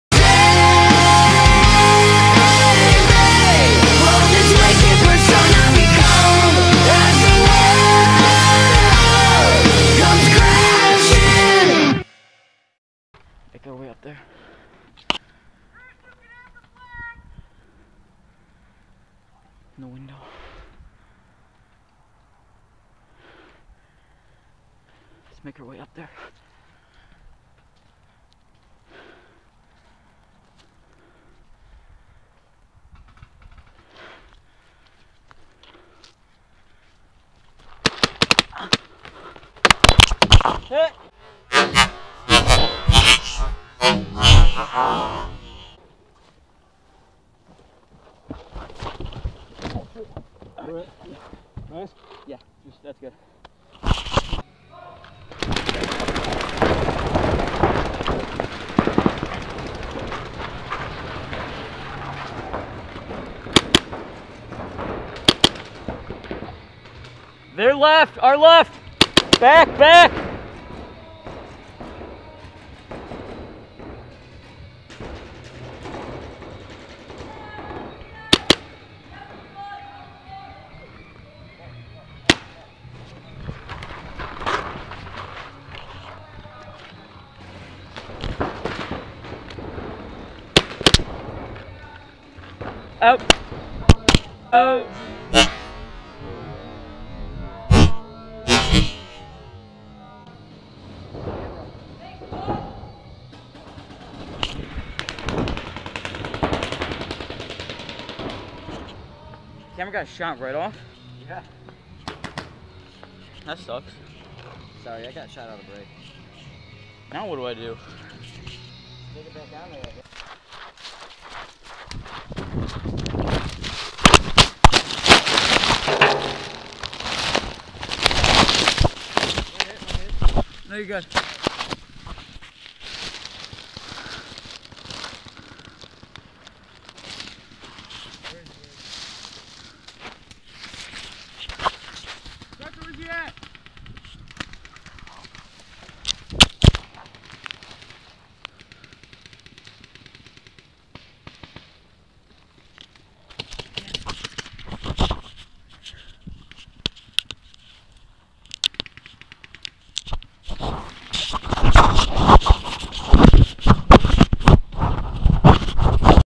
How Many Shots does it take to Shoot off Jacobs Camera?
this is a video of Jacobs camera getting shot off in games. hope you guys enjoy!